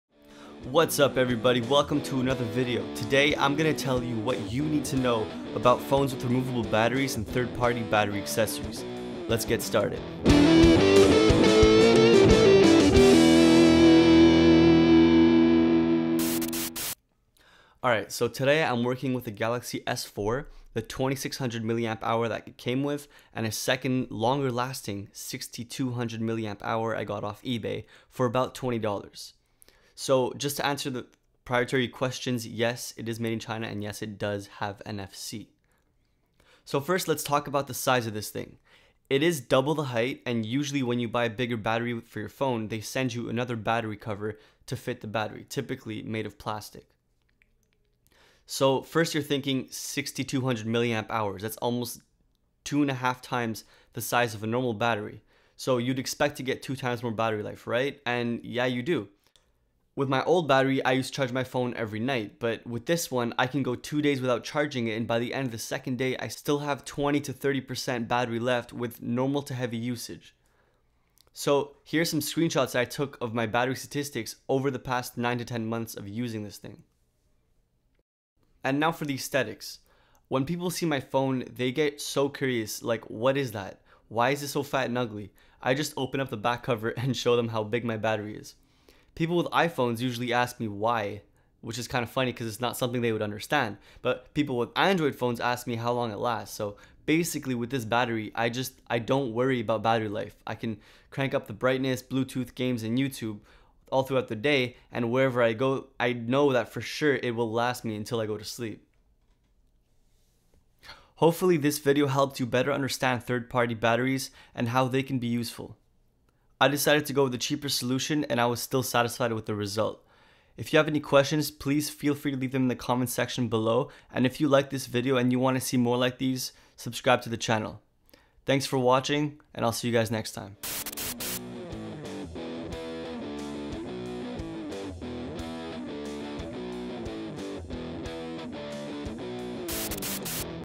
3rd Party Batteries Explained - HiTechJoint
In this video I will explain 3rd party batteries, their purpose and how well they performed for me.